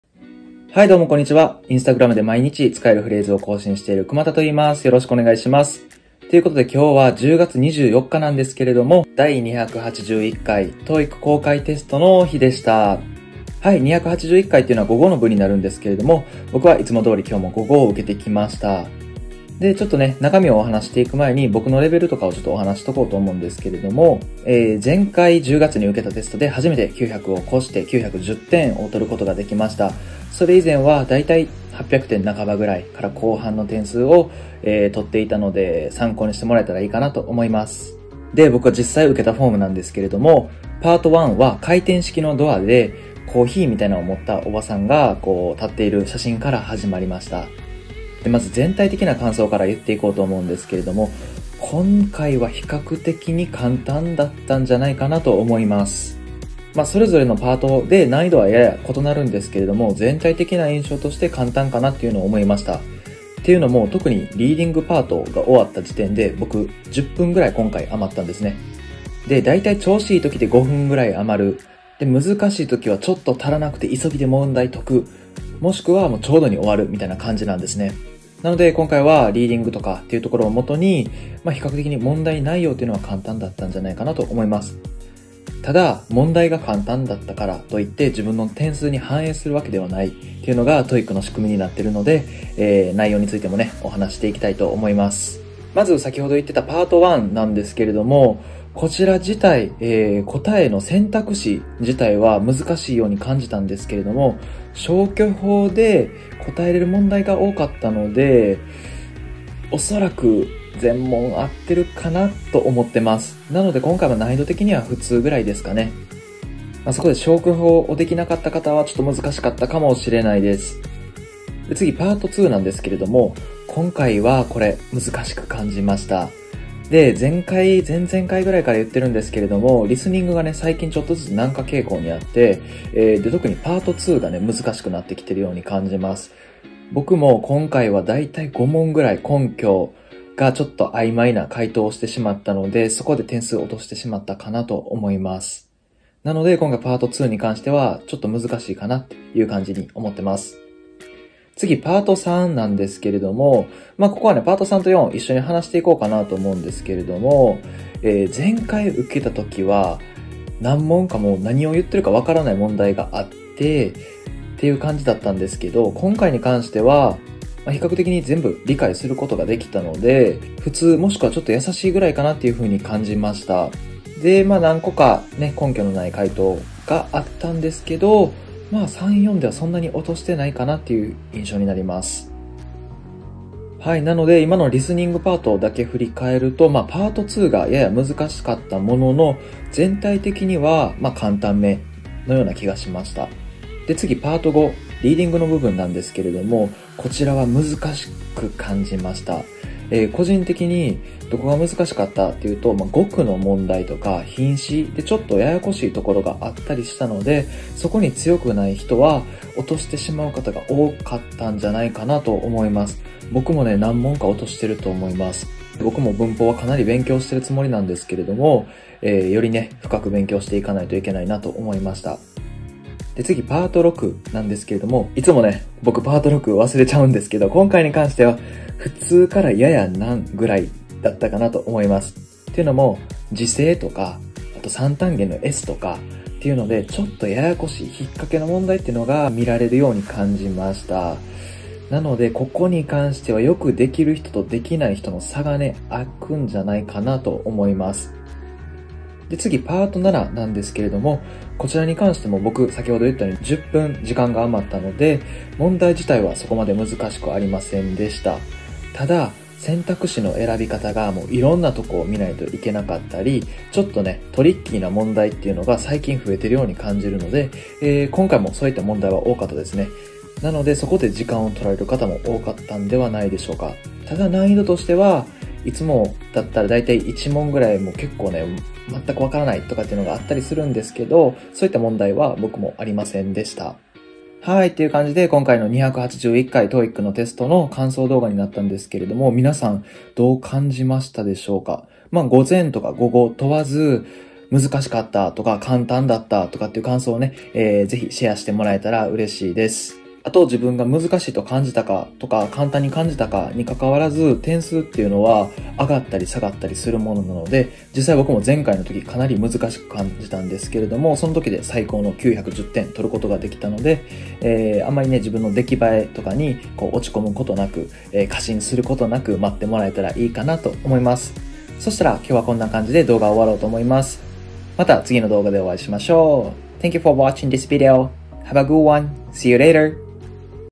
第281回 TOEIC公開テスト（10月24日 午後）テストの感想！ Part5が難しかった！？
こんにちは！くまたです！
普段はInstagramで英語のフレーズを投稿しています！

今回は　2021年10月24日の午後に受けた、第281回TOEIC公開テストの感想を話しています！参考にしてみてくださいね^_^

大学時代に英語の勉強の楽しさに目覚める。
社会人を経験後、退職してバンクーバーに半年間留学。